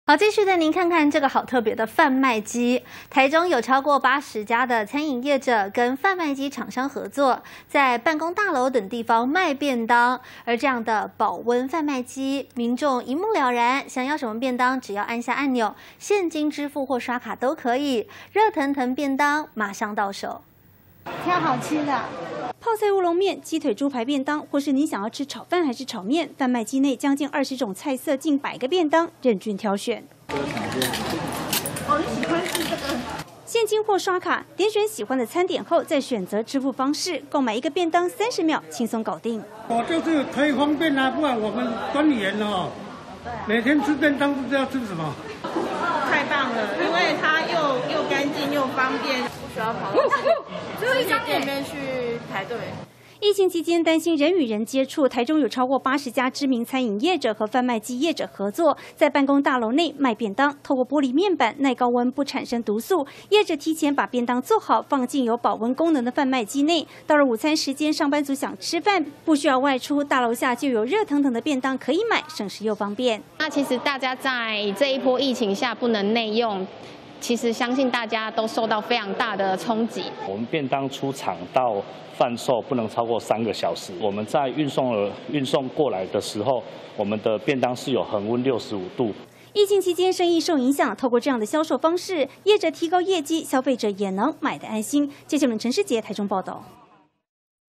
不用外送！便當保溫販賣機 隨看隨選馬上拿
疫情期間減少人與人的接觸，很多人會叫外送，或者現在您也可以選擇這種販賣機，台中有超過80家的餐飲業者和販賣機廠商合作，在各個辦公大樓等地方賣便當，保溫販賣機，利用玻璃面板，讓民眾一目了然，想要什麼樣口味的便當，按下按鈕，現金支付或是刷卡都可以，一個熱騰騰的便當馬上到手。